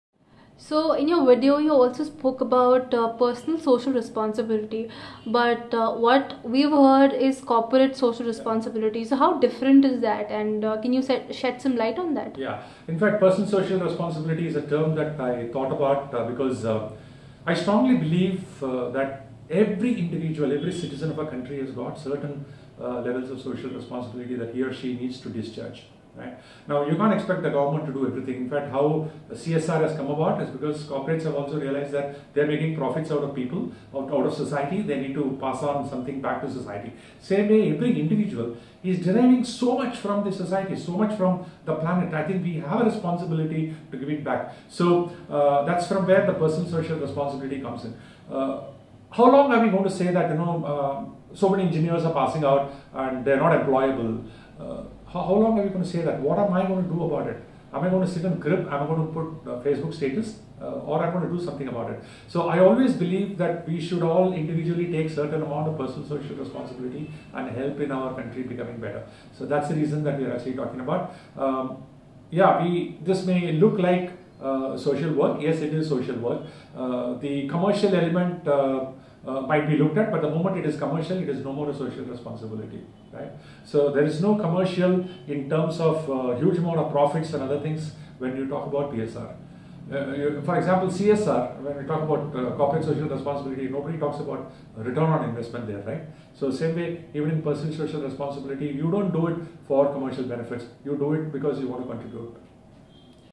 Personal Social Responsibility Versus Corporate Social Responsibility
Every individual derives a lot from the society, and now it's time that we give it back to the society. We should all take part in personal social responsibility and make our country a better one.